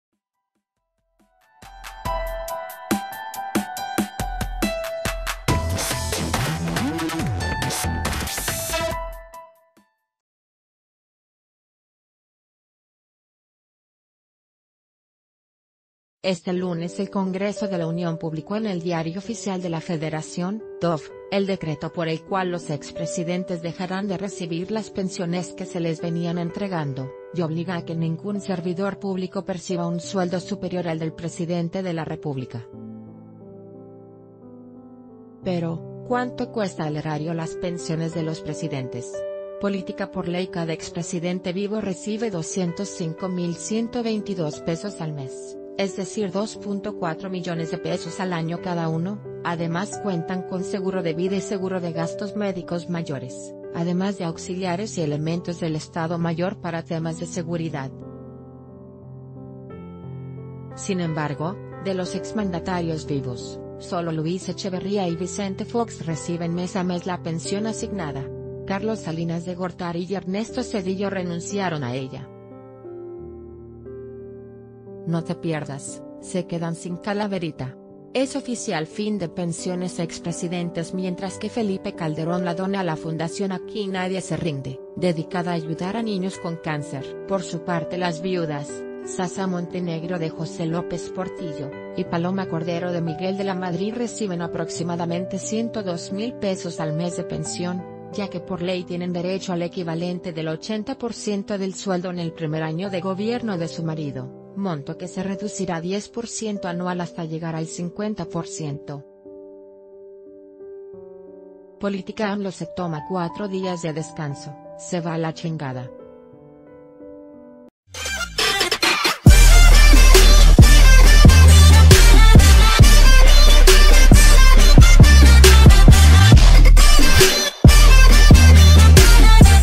¿Cuánto cobran los expresidentes por sus pensiones?
Este lunes el Congreso de la Uniónpublicó en el Diario Oficial de la Federación (DOF) el decreto por el cual los ex presidentes dejarán de recibir las pensionesque se les venían entregando y obliga a que ningún servidor público perciba un sueldo superior al del Presidente de la República. Pero ¿cuán...